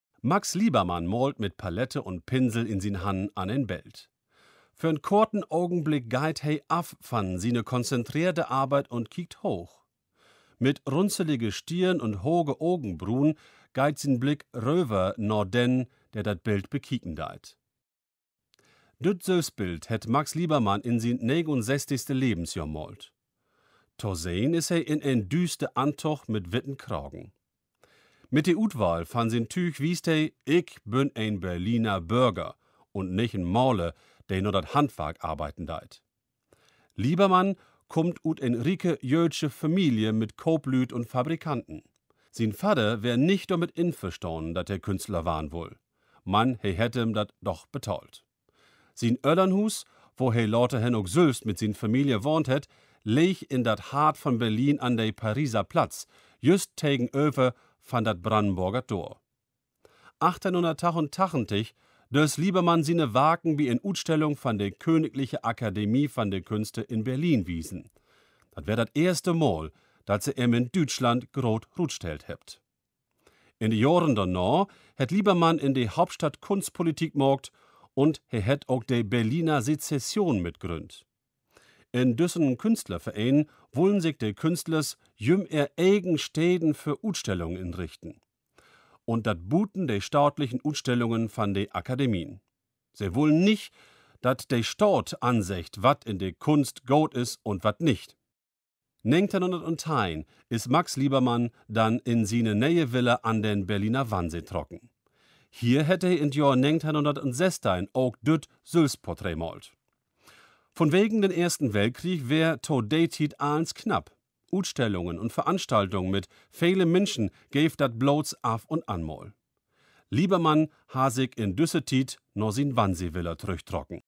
Max Liebermann Geburtstag
Unser heutiges Portrait ist dem deutschen Maler Max Liebermann gewidmet. Der bedeutende Impressionist wurde am 20. Juli 1847 in Berlin geboren. Gesprochen wird das Portrait auf Plattdeutsch von Ludger Abeln! Mehr plattdeutsche Texte findet ihr übrigens auf unserem kostenfreien Audioguide!